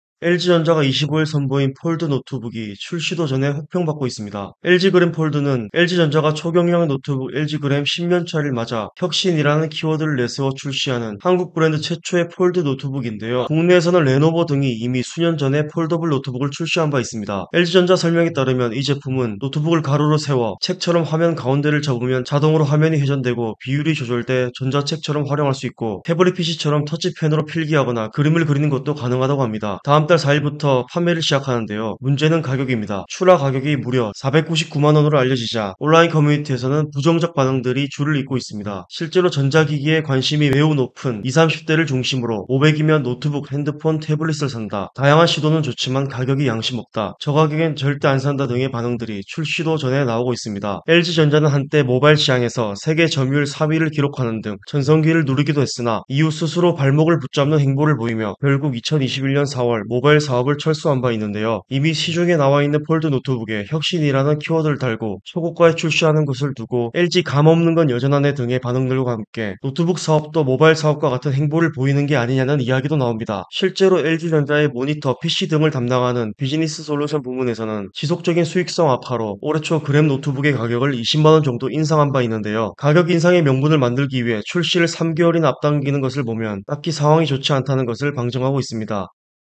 LG 그램 폴더블 노트북, 출시도 전에.."절대 안 산다"
🔷 기사 보기

LG전자가 25일 선보인 '폴드 노트북'이 출시도 전에 혹평받고 있습니다.

'LG 그램 폴드'는 LG전자가 초경량 노트북 'LG 그램' 10년 차를 맞아 '혁신'이라는 키워드를 내세워 출시하는 '한국 브랜드 최초'의 폴드 노트북인데요. 국내에서는 레노버 등이 이미 수년 전에 폴더블 노트북을 출시한 바 있습니다.

🔷 이포커스에 제보하기